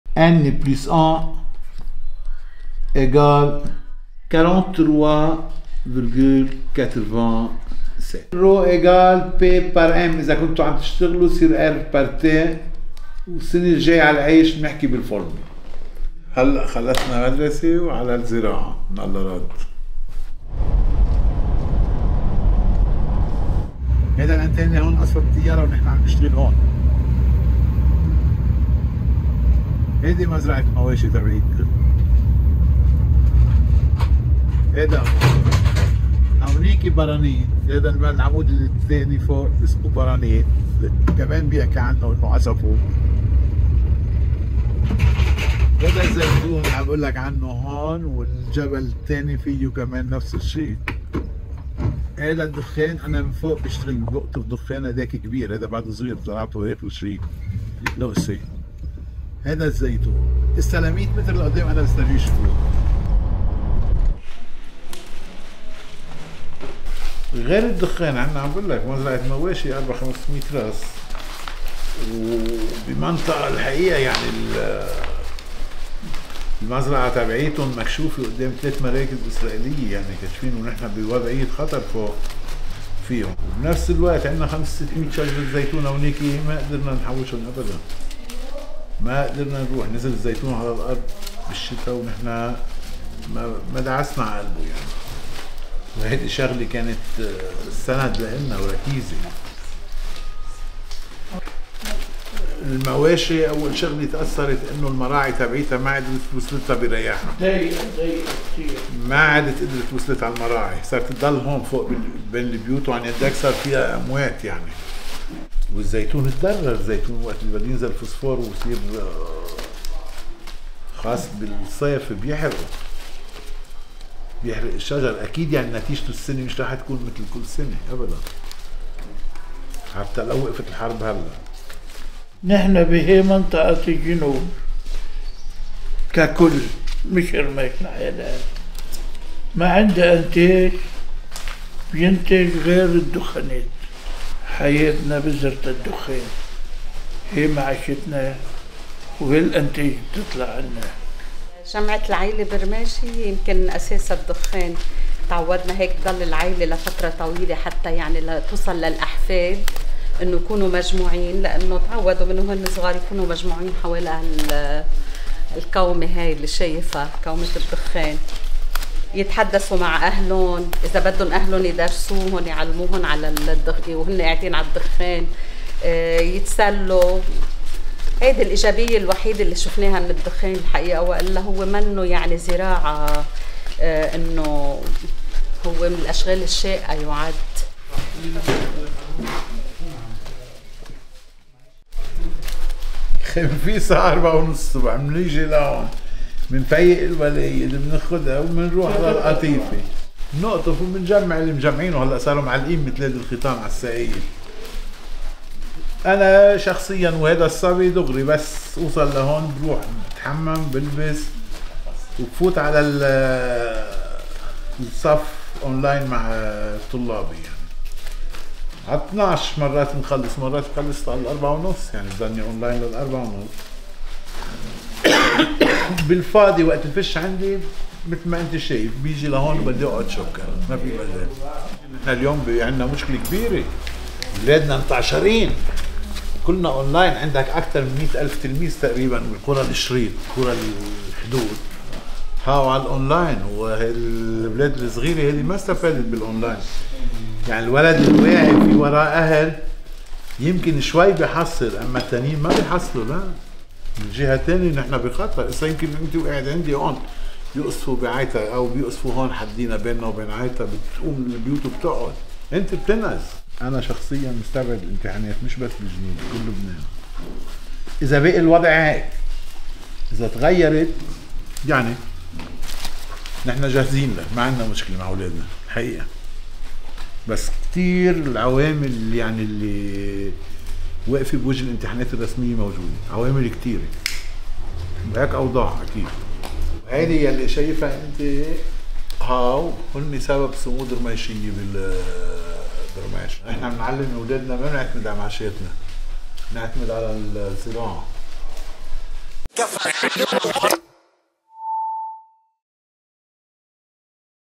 تعليمٌ «أونلاين» وزراعةُ التبغ في رميش الجنوبيّة الاستاذ طنّوس صامد في الجنوب
بعد ما يقارب 240 يوماً على بدء الاعتداءات الإسرائيلية على جنوب_لبنان، لا يزال في بلدة رميش الحدوديّة جنوبيّون صامدون رغم العدوان والدمار واستهداف المدنيّين. مواطنون يزرعون التبغ، يَدرُسون ويُدرِّسون «أونلاين»، تأكيداً على الثبات في الأرض.